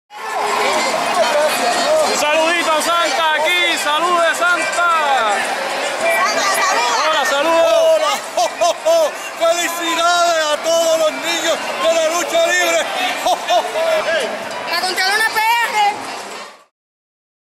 Santa Claus (Don David Ascencio) les desea una Feliz Navidad junto al Duende Ian
Descanse en paz Don David Ascencio.